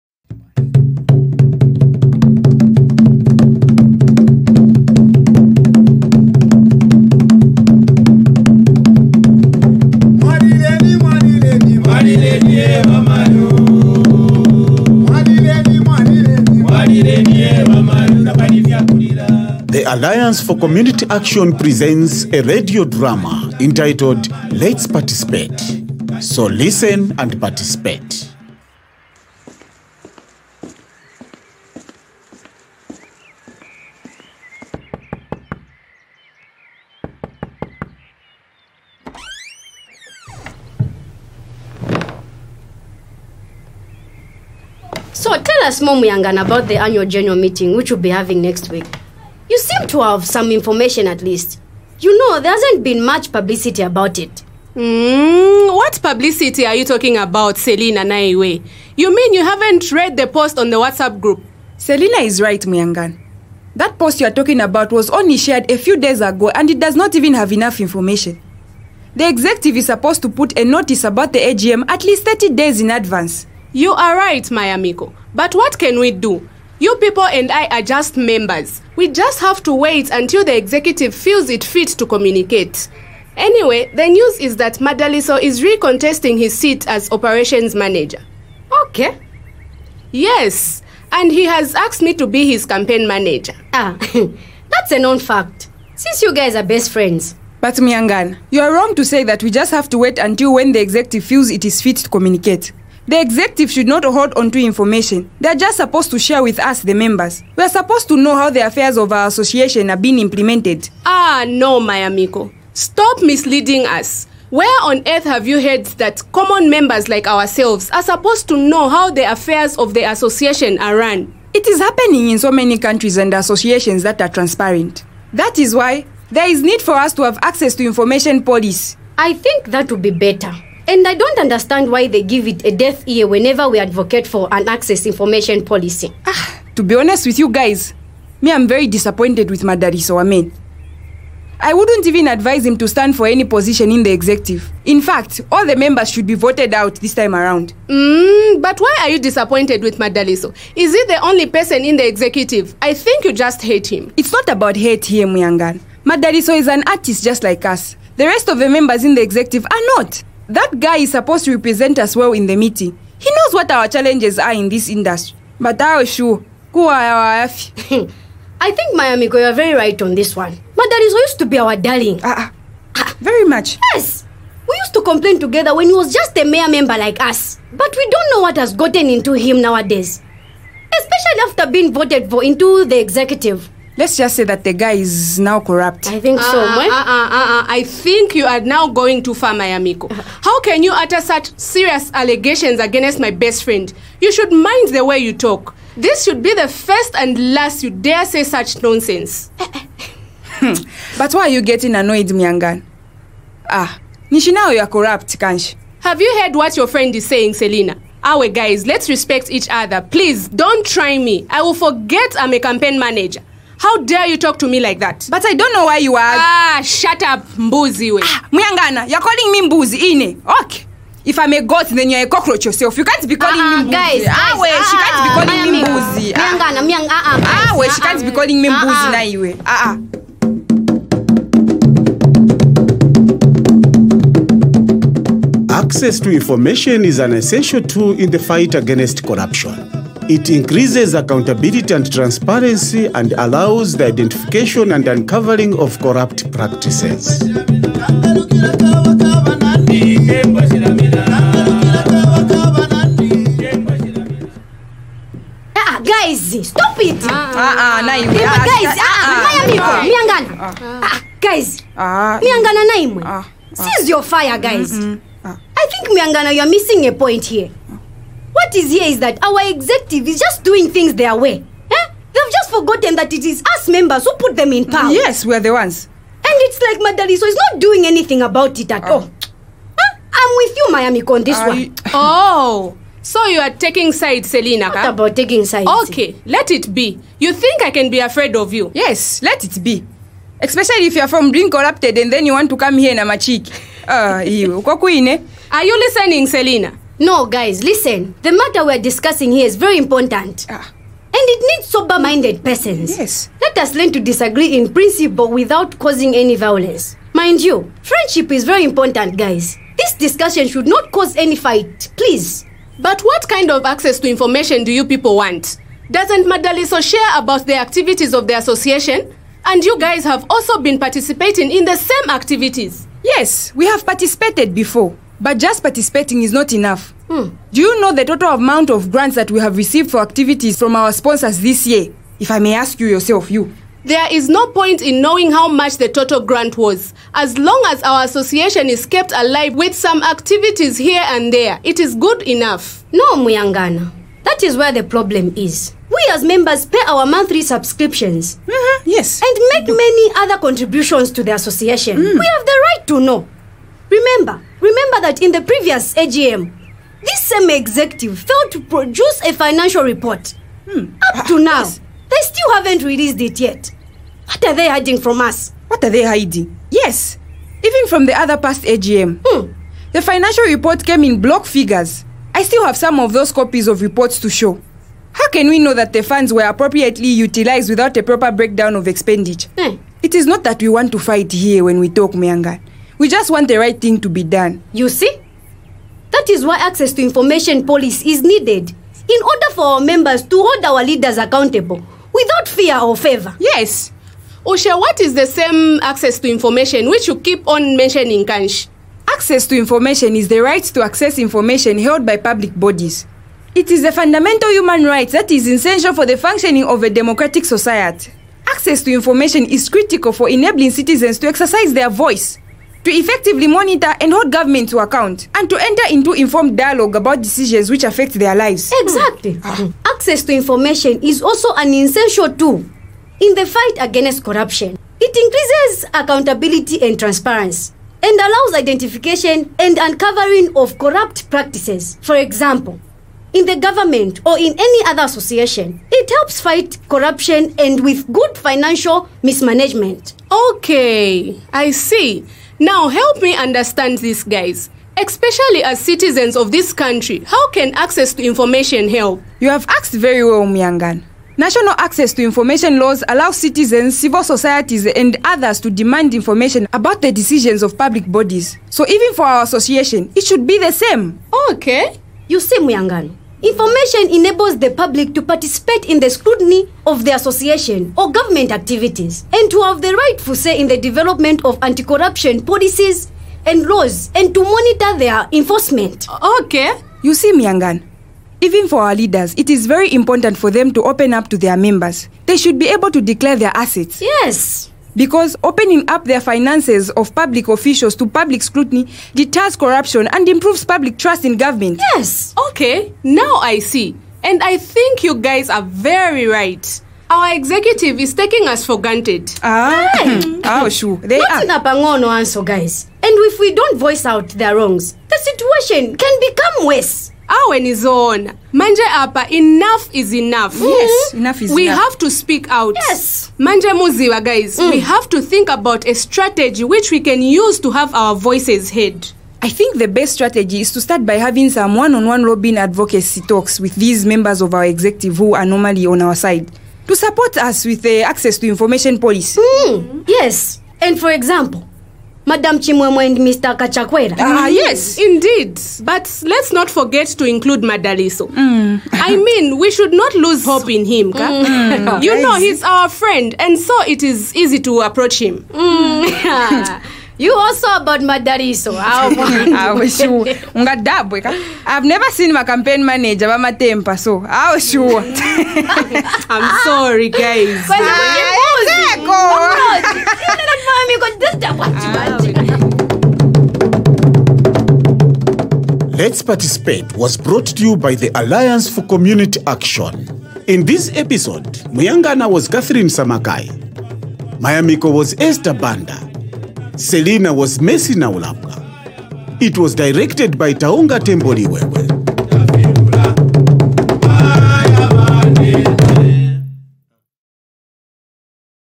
Access to Information and Corruption - English Audio Drama
In this episode, Barefeet Theatre looks at how Access to Information can help citizens fight corruption and financial mismanagement in Zambia.

Digital Ask is supported by HIVOS, People Unlimited.